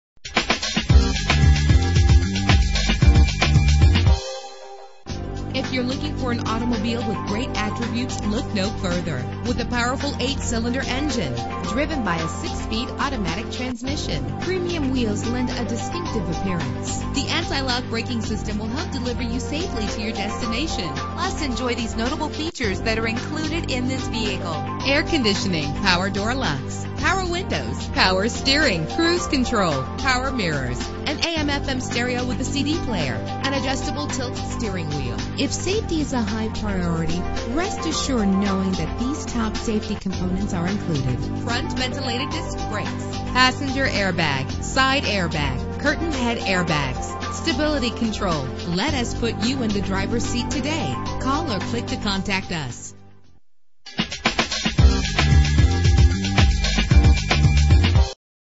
2012 Ram 1500 - Marietta GA
Year: 2012
Make: Ram
Model: 1500
Trim: SLT
Engine: 5.7 liter 8 cylinder 16 valve
Transmission: 6-Speed Automatic
Color: True Blue
Mileage: 10
Address:
789 Cobb Parkway